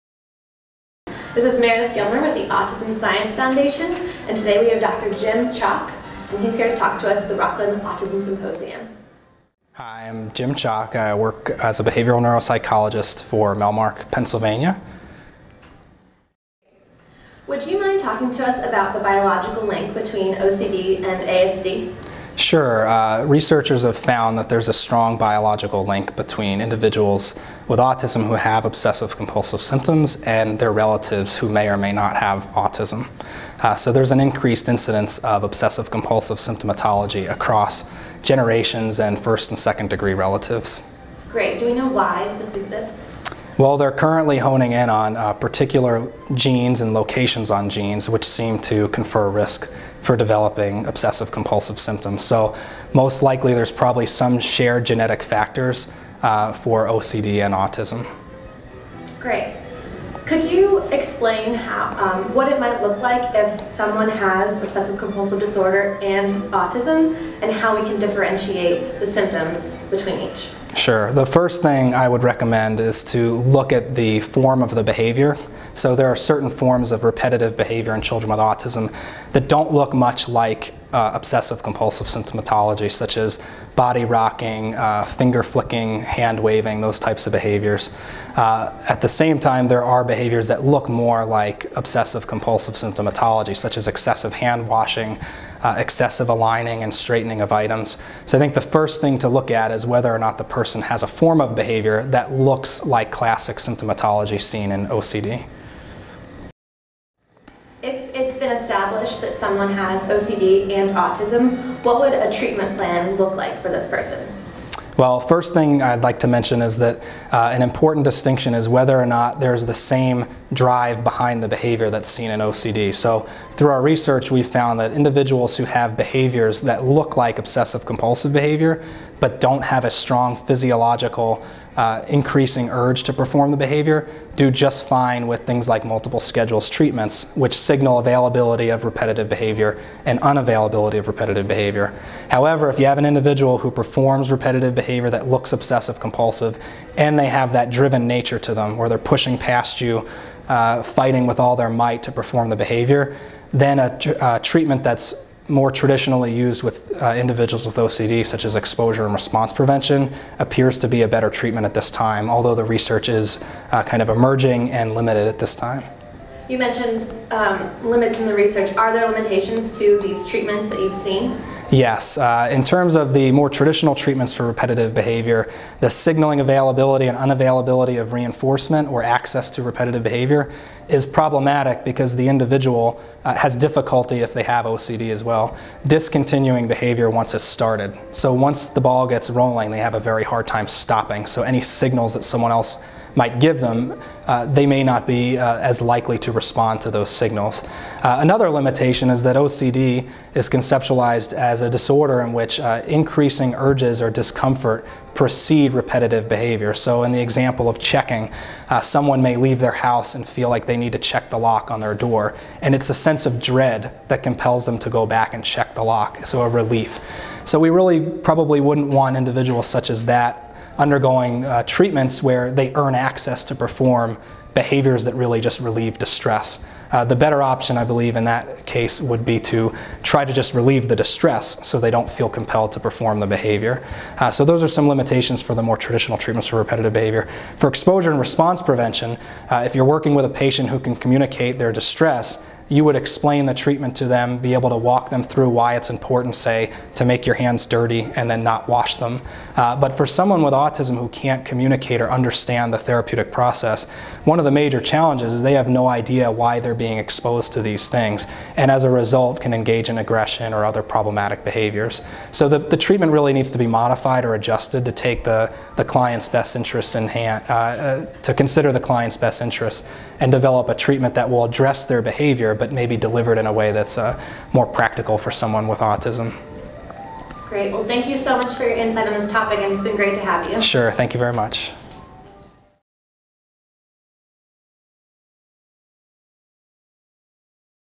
Dr. James Chok at the Rockland Autism Symposium
James Chok, Ph. D., BCBA-D, is a licensed psychologist, neuropsychologist, and Board Certified Behavior Analyst. Dr. Chok received his doctoral degree from the University of North Carolina and completed a one-year internship and two year postdoctoral fellowship in neuropsychology at McLean Hospital/Harvard Medical School. He is currently a Behavioral Psychologist/Neuropsychologist at Melmark New England. Dr. Chok is also the Director of Training for Massachusetts School of Professional Psychology practicum at Melmark New England. He has authored several papers in peer-reviewed scientific journals and presented research findings at numerous professional conferences. In 2011, Dr. Chok was the recipient of the Outstanding Practitioner Award given by the Berkshire Association for Applied Behavior Analysis and Therapy. He is currently investigating the assessment and treatment of obsessive-compulsive behavior in individuals with autism spectrum disorders, teaches courses in research design and measurement at UMASS Boston and Endicott College, and is the Vice President of the New Hampshire chapter of the International OCD Foundation.